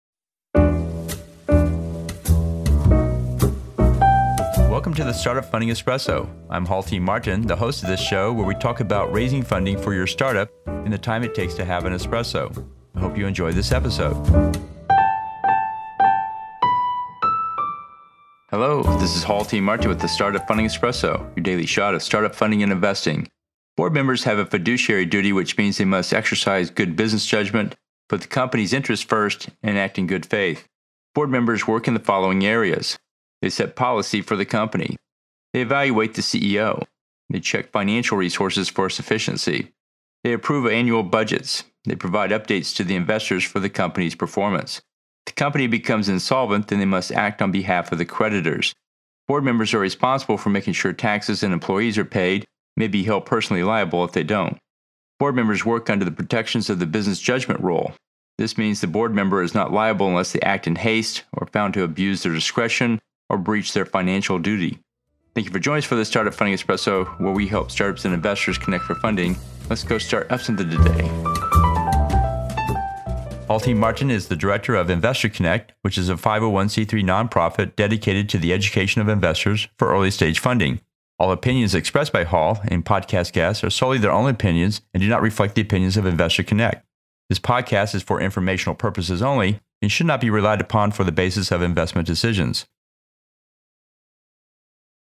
Startup Funding Espresso -- Board Fiduciaries
Board members have a fiduciary duty which means they must exercise good business judgment, put the company’s interest first, and act in good faith.

 Board members work in the following areas:

  They set policy for the company Evaluate the CEO Check the financial resources for sufficiency Approve annual budgets Provide updates to the investors for the company’s performance  If the company becomes insolvent then they must act on behalf of the creditors.

 Board members are responsible for making sure taxes and employees are paid and may be held personally liable if they don’t.

 Board members work under the protection of the business judgment rule.

 This means the board member is not liable unless they act in haste, are found to abuse their discretion, or breach their financial duty.

Thank you for joining us for the Startup Funding Espresso where we help startups and investors connect for funding.Let’s go startup something today.___________________________________For more episodes from Investor Connect, please visit the